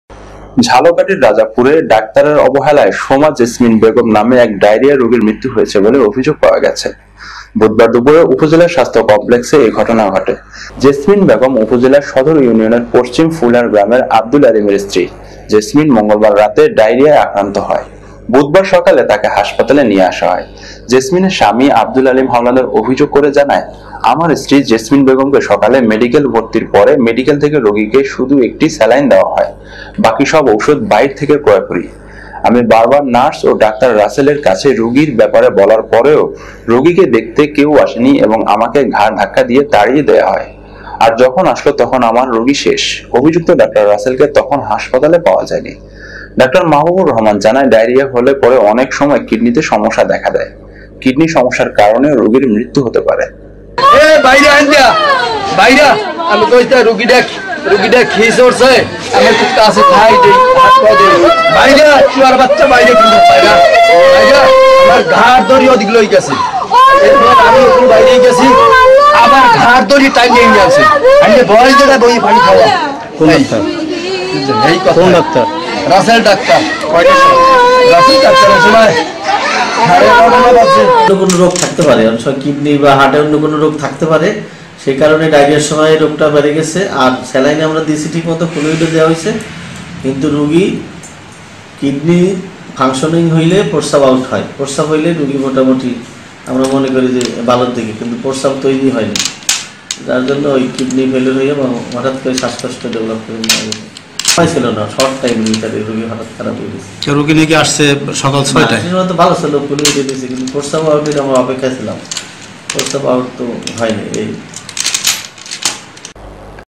ঝালকাঠির রাজাপুরে ডাক্তারের অবহেলায় রোগী মৃত্যুর অভিযোগ|Jhalokathi|71Bangla TV
ঝালকাঠির রাজাপুরে ডাক্তারে অবহেলায় সোমাঃ জেসমিন বেগম (৪০) নামে এক ডায়রিয়ার রোগীর মৃত্যু হয়েছে বলে অভিযোগ পাওয়া গেছে। বুধবার দুপুরে উপজেলা স্বাস্থ্য কমপ্লেক্সে এ ঘটনা ঘটে। জেসমিন বেগম উপজেলার সদর ইউনিয়নের পশ্চিম ফুলহার গ্রামের আব্দুল আলীমের স্ত্রী। জেসমিন মঙ্গলবার রাতে ডায়রিয়ায় আক্রান্ত হয়।বুধবার সকালে তাকে হাসপাতালে নিয়ে আসা হয়।
জেসমিনের স্বামী আব্দুল আলীম হাওলাদার অভিযোগ করে জানায়, আমার স্ত্রী জেসমিন বেগমকে সকালে মেডিকেলে  ভর্তির পরে মেডিকেল থেকে রোগীকে শুধু একটি স্যালাইন দেয়া হয়। বাকি সব ঔষধ বাহির থেকে ক্রয় করি। আমি বার বার নার্স ও ডাক্তার রাসেল এর কাছে রোগীর ব্যাপারে বলার পরেও রোগীকে দেখতে কেউ আসেনি এবং আমাকে ঘার ধাক্কাদিয়ে তাড়িয়ে দেয়। আর যখন আসলো তখন আমার রোগী শেষ। অভিযুক্ত ডাঃ রাসেলকে তখন হাসপাতালে পাওয়া যায়নি। ডাঃ মাহবুবুর রহমান জানান, ডায়রিয়া হলে পরে অনেক সময় কিডনিতে সমস্যা দেখা দেয়। কিডনি সমস্যার কারনেও রোগি মৃত্যু হতে পারে।